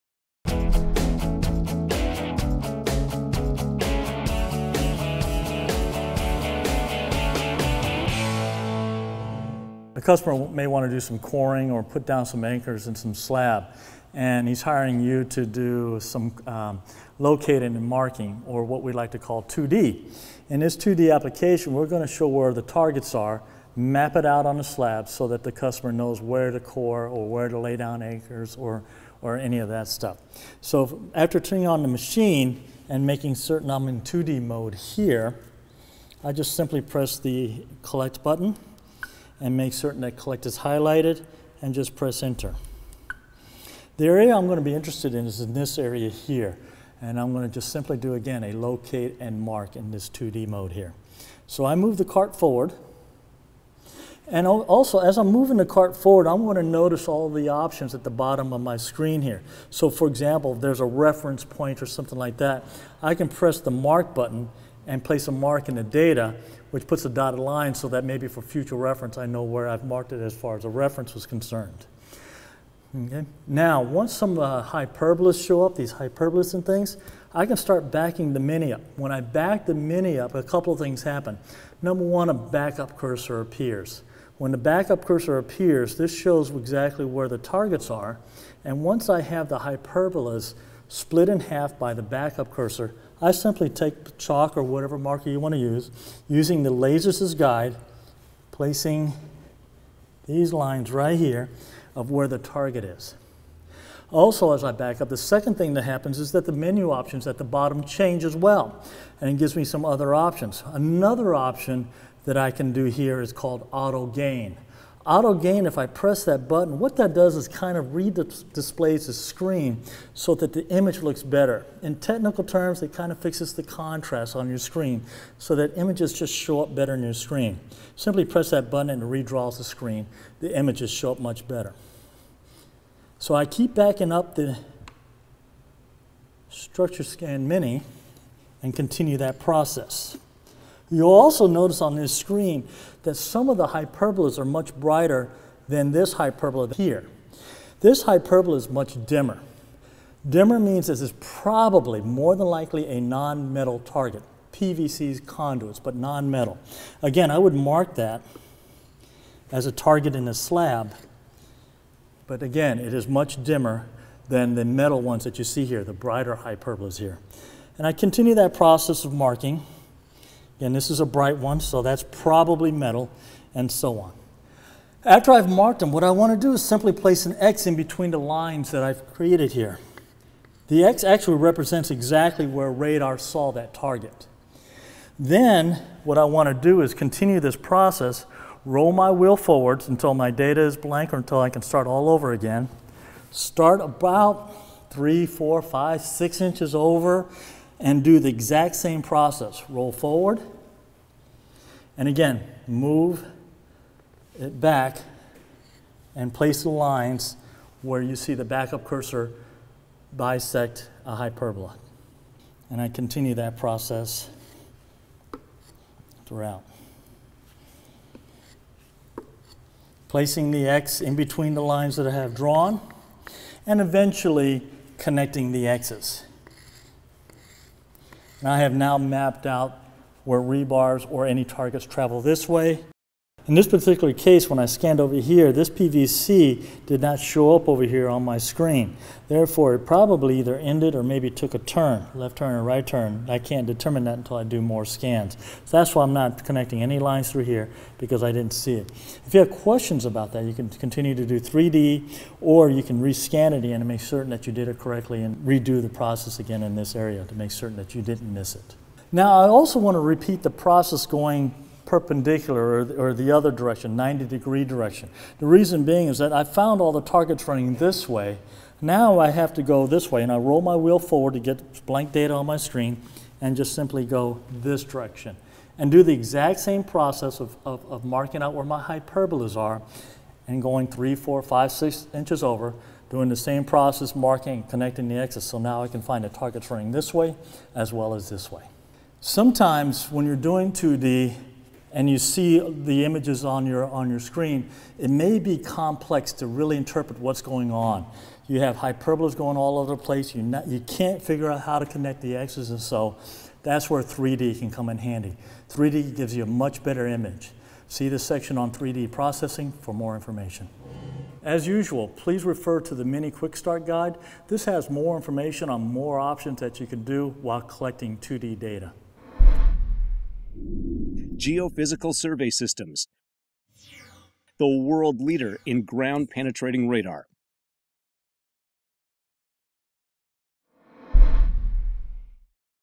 How to: Locate Rebar in Concrete using Ground Penetrating Radar
In this video, we demonstrate how to locate and mark rebar and conduit in a concrete slab. This example illustrates real-time 2D scanning to identify embedded objects and features, and how to mark them in your survey area.